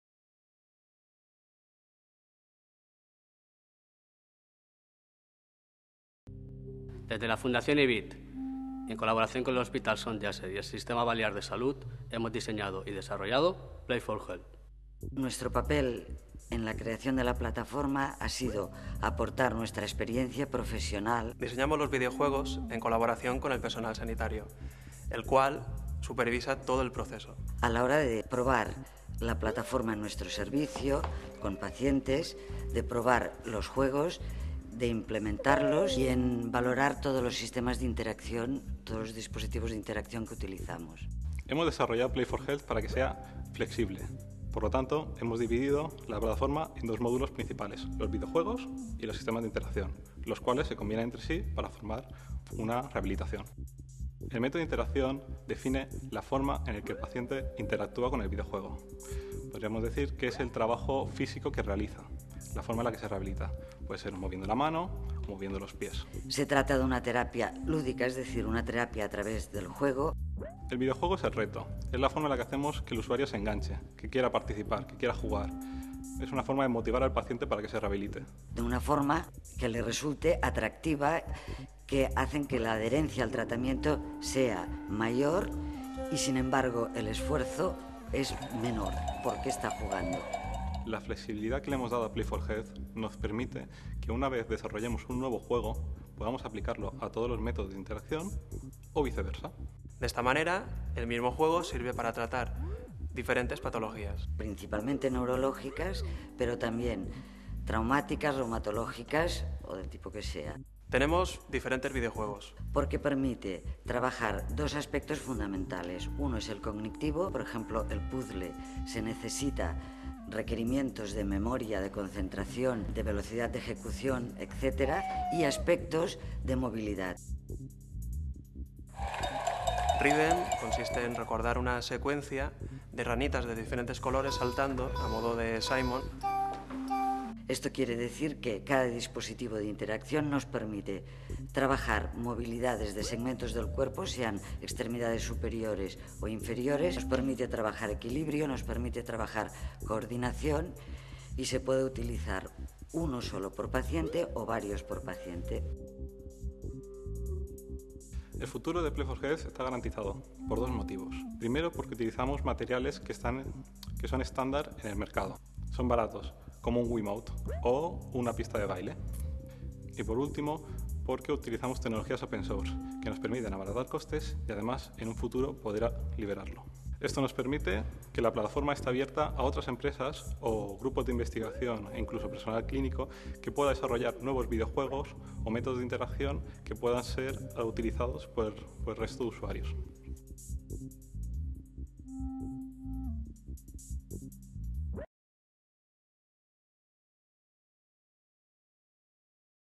P4H - Plataforma- Rápido de implementar. Fácil de usar
Play for Health agiliza el desarrollo y despliegue de proyectos de telerehabilitación de una forma homogénea para todo el servicio público de salud. Hemos desarrollado la plataforma P4H en colaboración con los profesionales sanitarios, que supervisan el diseño y la adecuación a cada terapia.